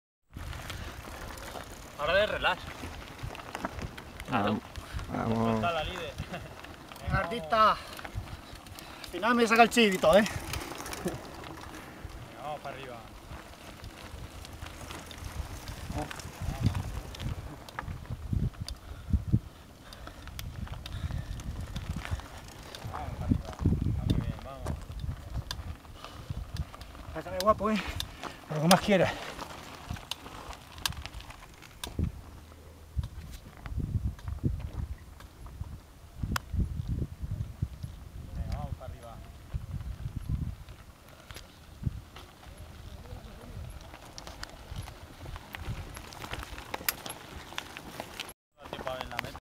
BTT FUENTEÁLAMO 2010.PASO KM 30 IV.PRODEPOR
PASO POR EL KILÓMETRO 30 DE GRUPO PERSEGUIDOR III, EN LA PRUEBA DIPSUTADA EN FUENTEÁLAMO DENTRO DEL IV CIRCUITO BTT DIPUTACIÓN DE ALBACETE.